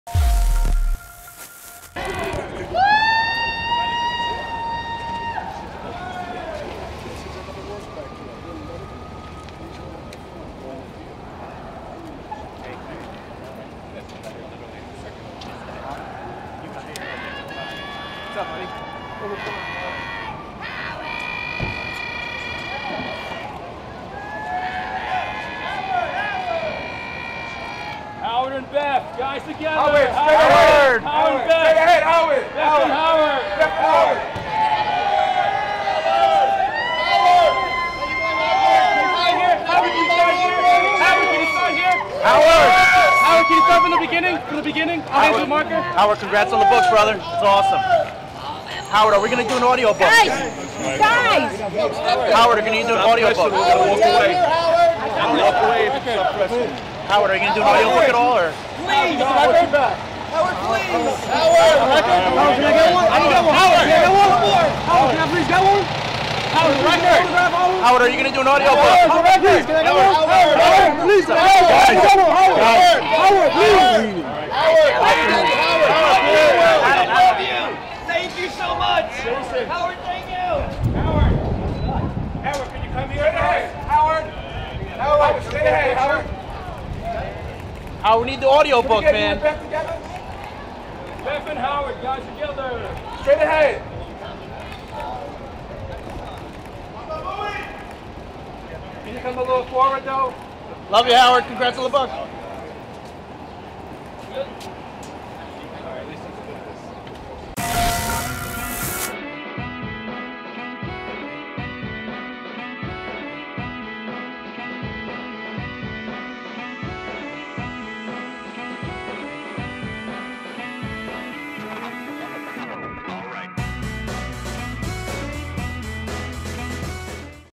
Howard Stern Creates Fan Frenzy Outside Stephen Colbert Show
Howard Stern is on a whirlwind book promotion tour, and his devoted fans have been making sure his name is shouted through the streets of NYC.

The King of All Media, and his wife Beth Stern, arrived Tuesday to tape the “The Late show with Stephen Colbert,” and they were greeted by a crowd of excited fans waiting for autographs.